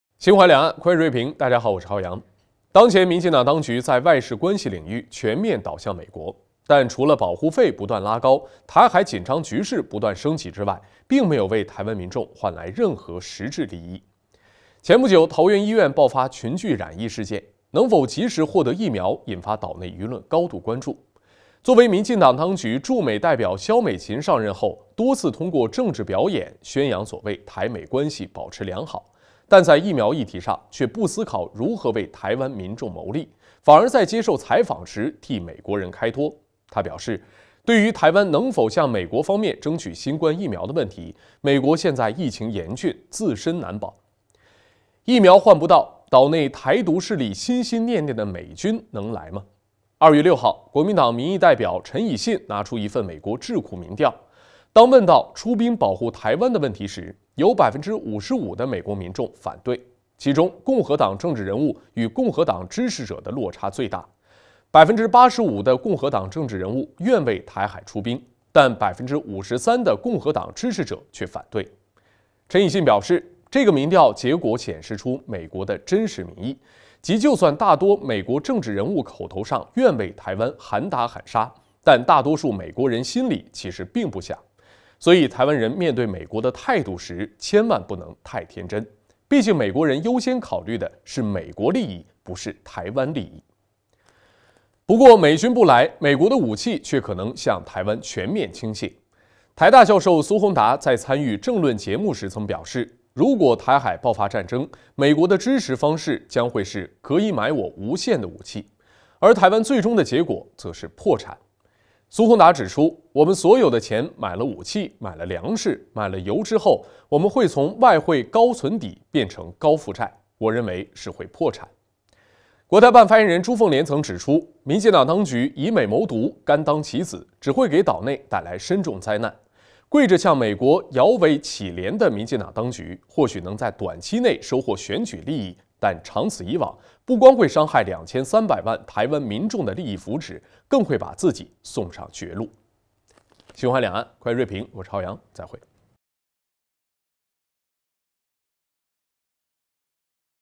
民進黨當局全面倚美謀「獨」換不來疫苗，台海開戰時更換不來美軍！
兩岸快評 跪著向美國搖尾乞憐的民進黨當局，或許能在短期內收穫選舉利益，但長此以往，不光會傷害2300萬台灣民眾的利益福祉，更會把自己送上絕路。